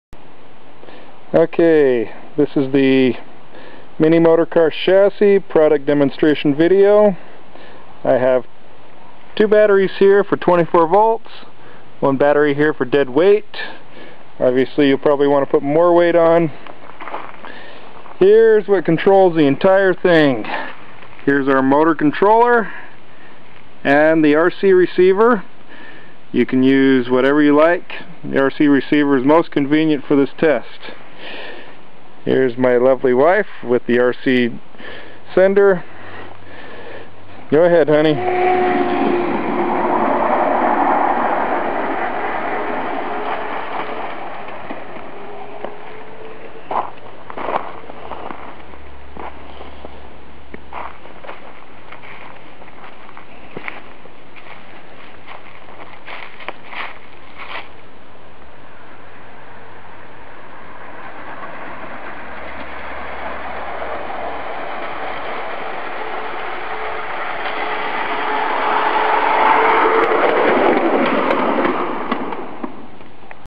Power chassis
Testing a Ride Trains mini motor car power chassis, single 350 watt motor.